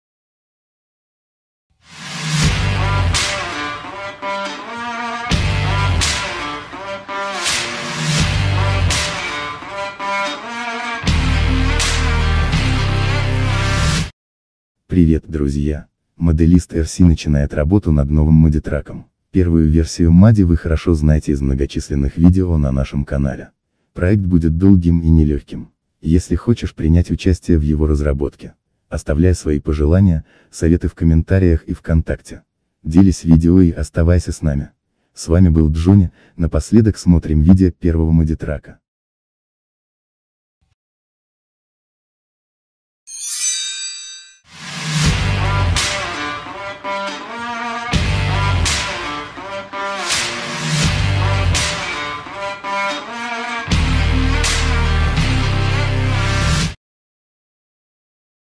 NEW!!! MUDDY II
ModelistRC.ru
ModelistRC строит новую модель МАДДИТРАКА (MUDDY TRUCK), хочешь узнать как будет она выглядеть, смотри это видео. Хочешь принять участие в ее разработки оставляй идеи в комментах!